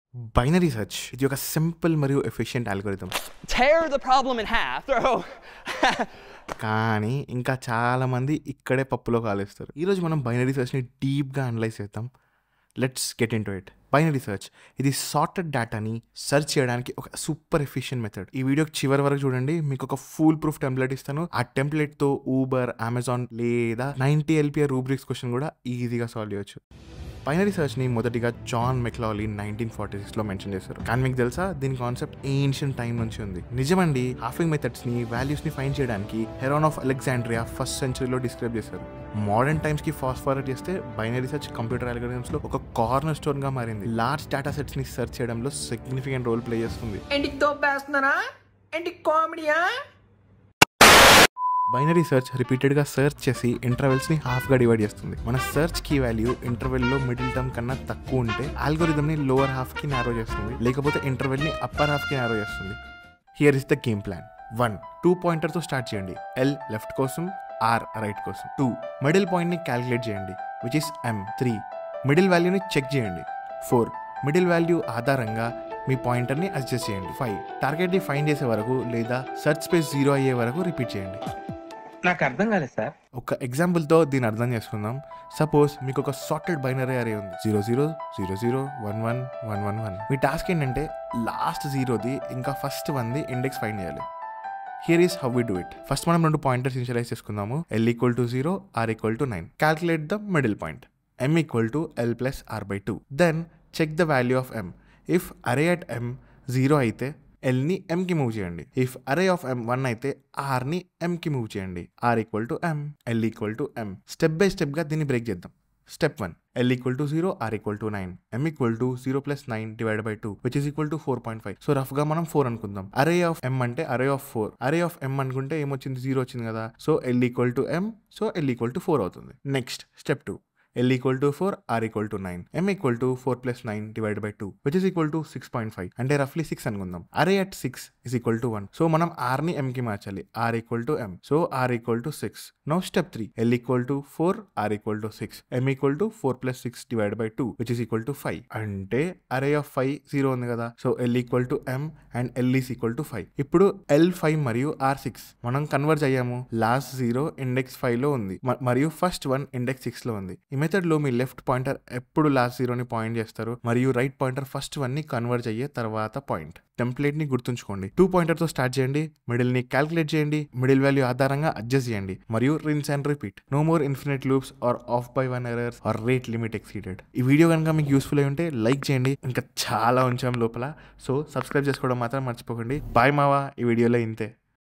Binary Search Explained In Telugu
నారేటర్ (సజీవ బ్యాక్గ్రౌండ్ సంగీతంతో):
Binary Search! ఇది ఒక చాలా సరళమైన మరియు ప్రభావవంతమైన Algorithm, కాని చాలా మంది students కు ఇది మరింత చేదు. ఈ రోజు, మేము Binary Search ని లోతుగా విశ్లేషిద్దాం. రండి, Binary Search గురించి తెలుసుకుందాం - ఇది క్రమబద్ధమైన డేటాను search చేయడానికి చాలా ప్రభావవంతమైన పద్ధతి.
ఈ వీడియో చివరలో, మీకు Binary Search కోసం ఒక అద్భుతమైన template ఉంటుంది.
Binary Search ని మొదట John Mauchly 1946 లో పేర్కొన్నారు, కాని మీకు తెలుసా ఈ ఆలోచన ప్రాచీన కాలం నుండి ఉందని? అవును, Heron of Alexandria 1వ శతాబ్దంలో విలువల్ని కనుగొనడానికి "halving method" ను వివరించారు!
ప్రస్తుత కాలానికి వెళ్లుదాం, Binary Search computer algorithms లో ఒక cornerstone గా మారింది, large datasets ను search చేయడంలో ప్రధాన పాత్ర పోషిస్తుంది.
Binary Search అనేది ప్రాంతాన్ని repeatedly divide చేస్తూ, ప్రాంతాన్ని half గా విభజిస్తుంది. search key value ప్రాంతం లోని మధ్య item కంటే తక్కువగా ఉంటే, algorithm కనిష్ట భాగాన్ని focus చేస్తుంది. లేకపోతే, ఆ ప్రాంతాన్ని upper half కు focus చేస్తుంది.
ఇక్కడ plan:

1. Start with two pointers, L for left and R for right.
2. Calculate the middle index, M.
3. Check the value at M.
4. Adjust the pointers based on the middle value.
5. Repeat the process until you find the target or the search space becomes zero.

dsa bro binary search telugu